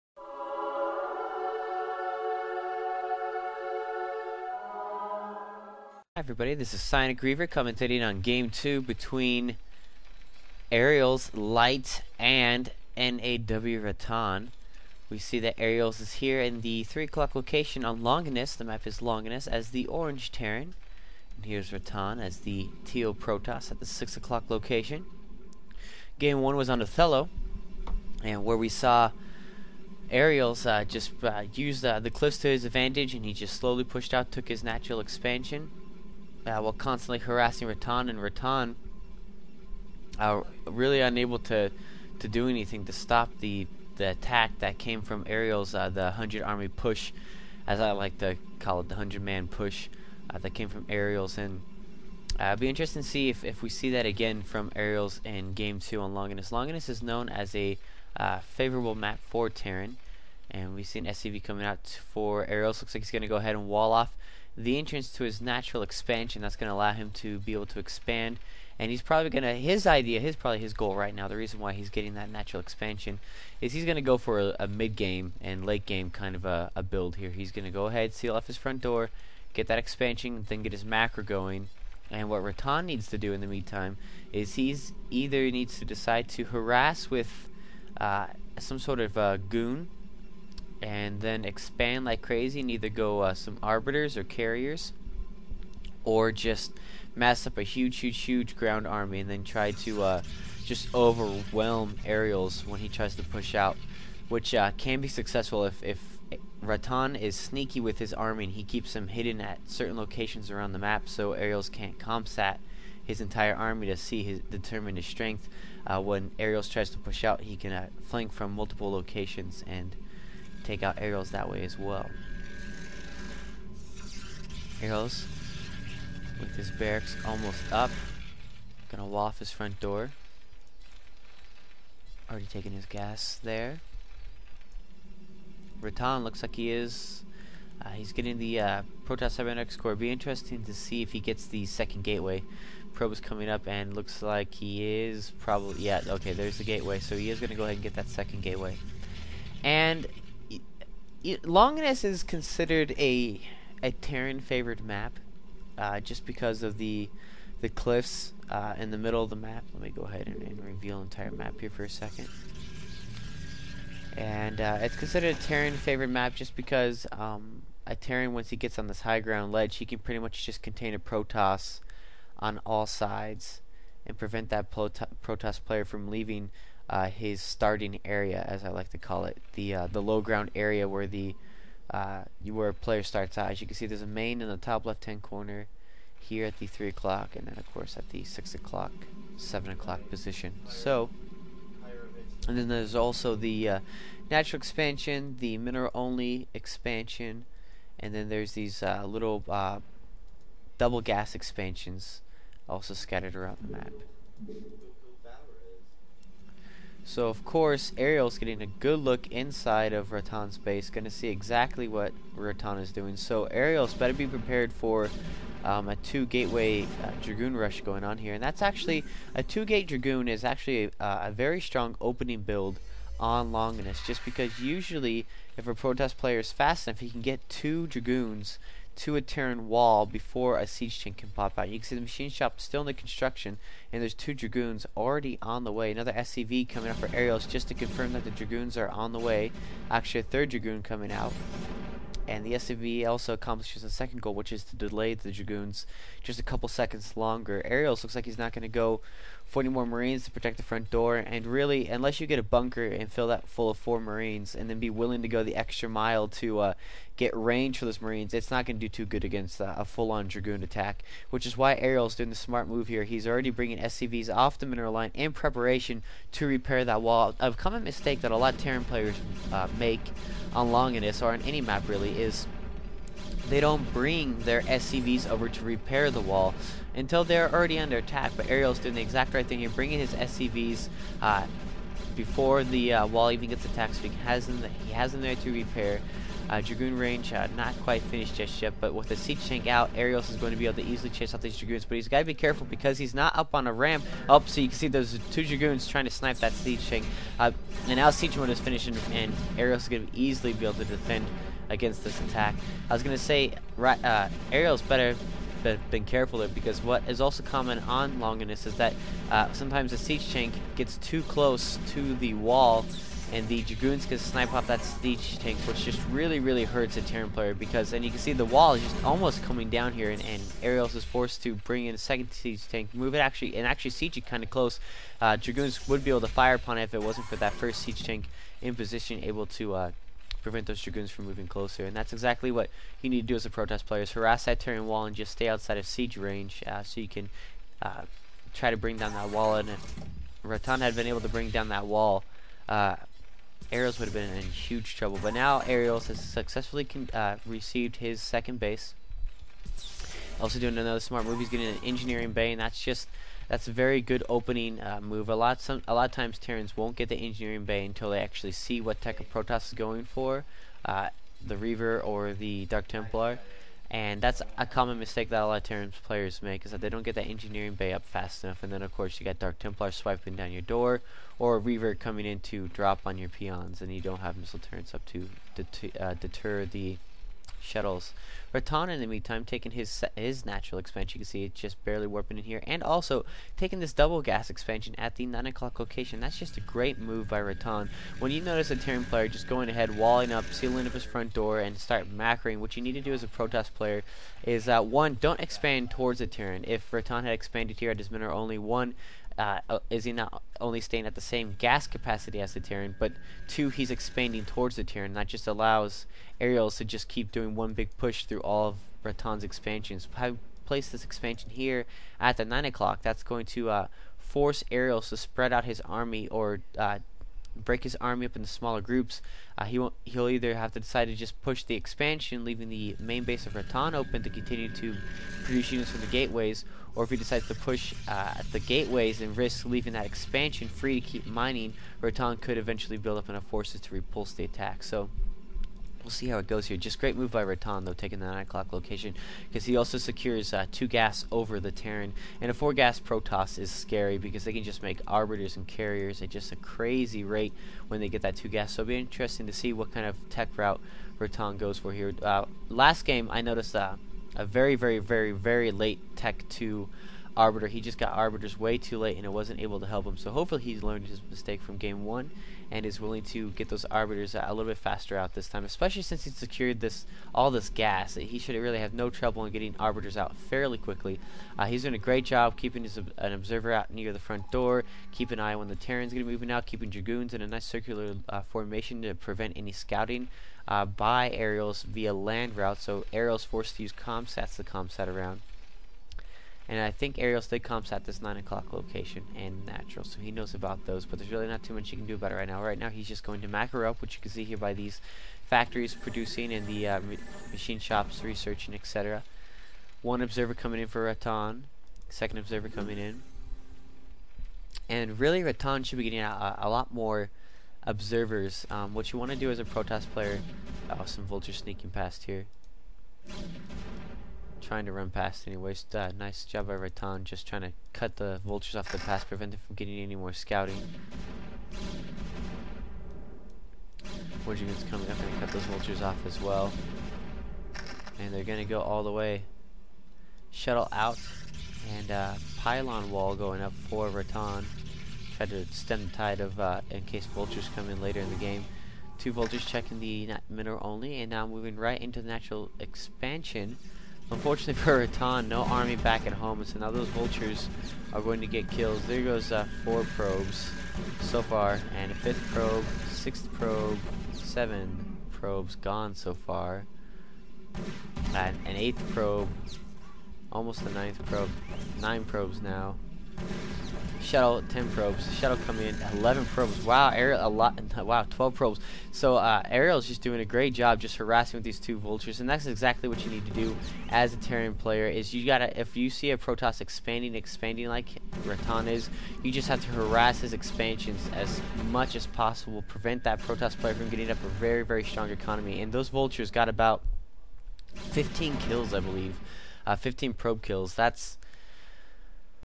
LighT KOTH #1 Game2: AeriALs vs RataN - July 26th, 2008 p1/3
Commentator: PsyonicReaver
Map: Longinus
Event: LighT KOTH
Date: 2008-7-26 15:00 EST
Player: Aerials[LighT] - T
Player: NaW-Ratan - P